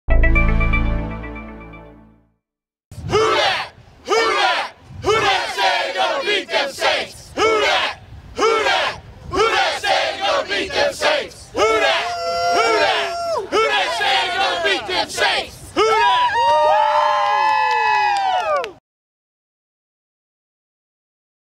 Who Dats chant as they tailgate in Charlotte
Saints fans from around the south chat before the start of the Saints game against the Panthers in Charlotte.